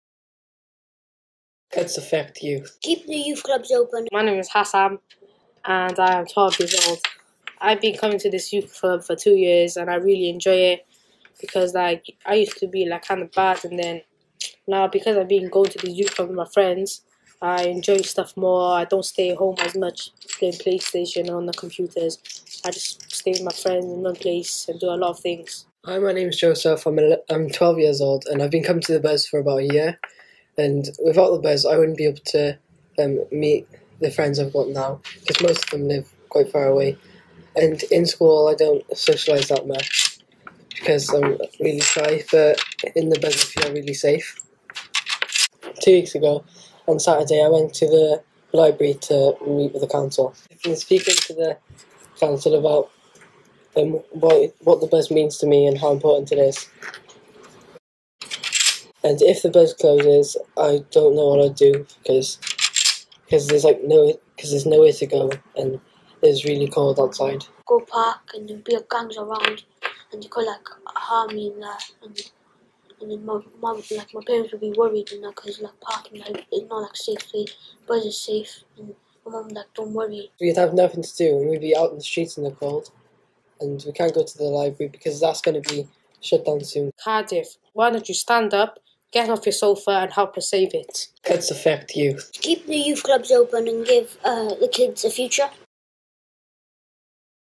CUTS AFFECT YOU[TH] @ theBUZZ Juniors
Views of a local young people voicing their concerns for the future of other young people and youth services in Cardiff.
If you have a voice then have a choice in sharing this video and others CUTS AFFECT YOU[TH]